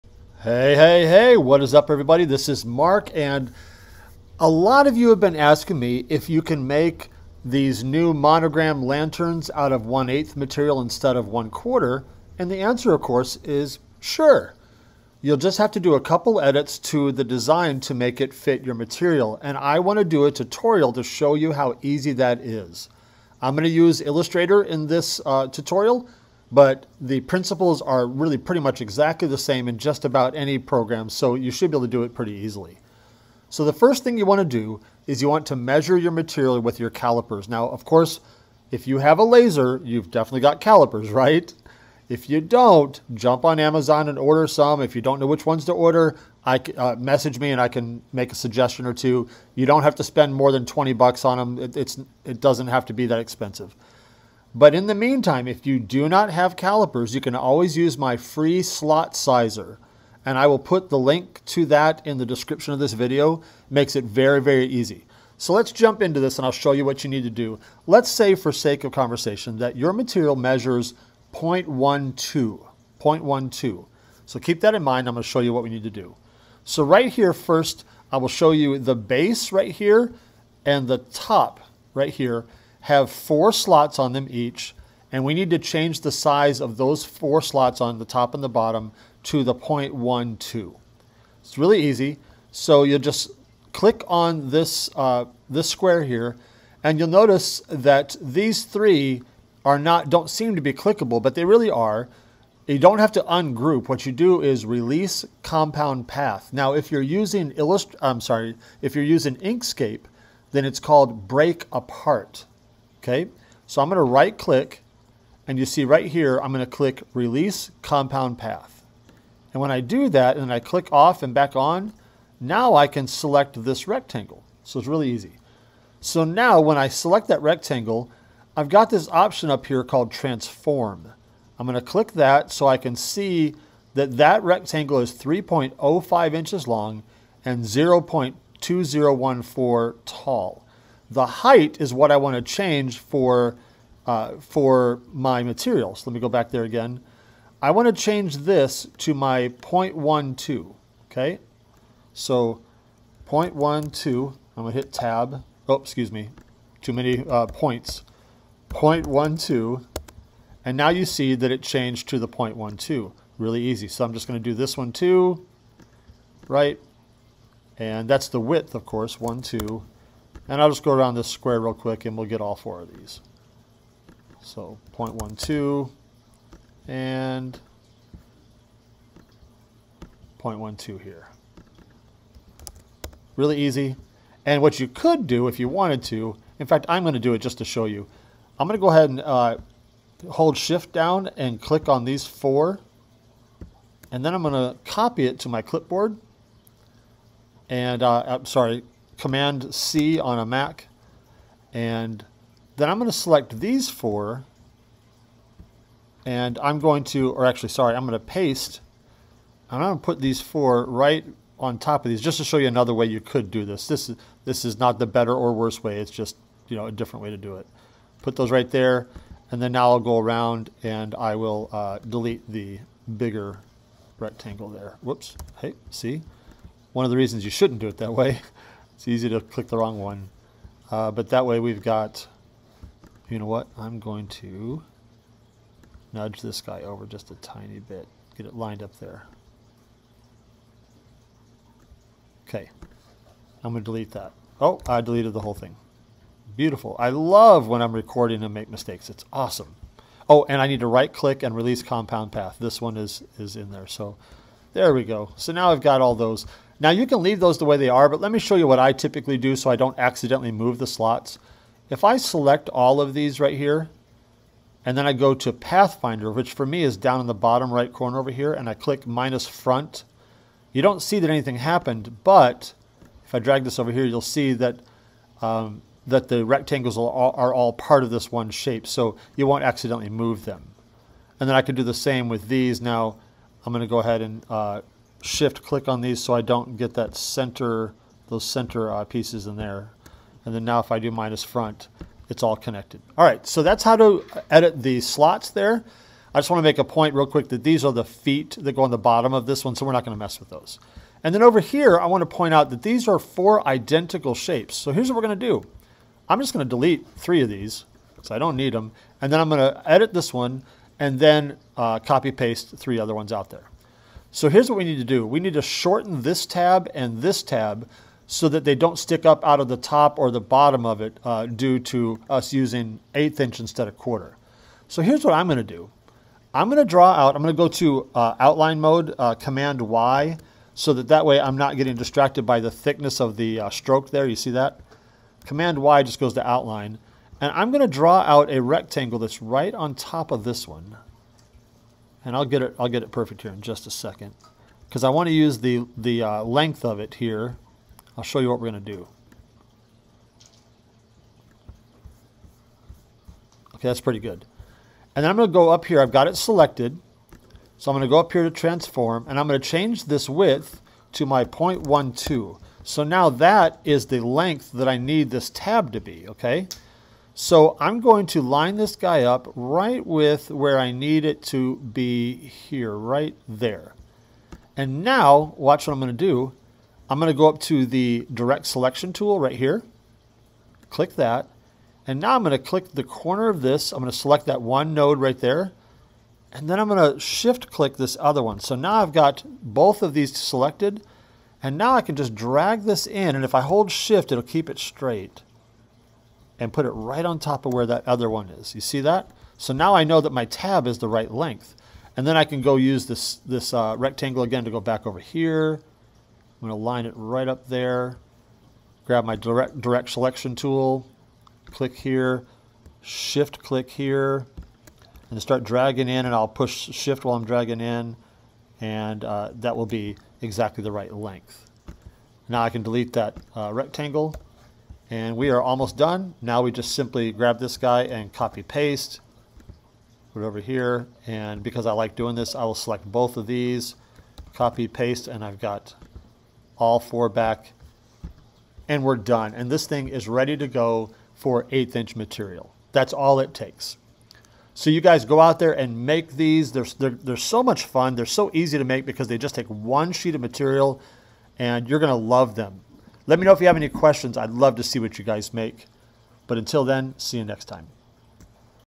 Tutorial: Editing Slots and Tabs for Different Material Thicknesses (Adobe Illustrator)
In this tutorial I show you how to edit the slots in my Monogram Lantern design that's originally for 1/4" material to work for 1/8" material. I am using Illustrator, but the principle will work easily for any vector program.

Mark // BigBlueLaserDesigns
My Mission: "To help beginning and busy laser owners create great things faster!"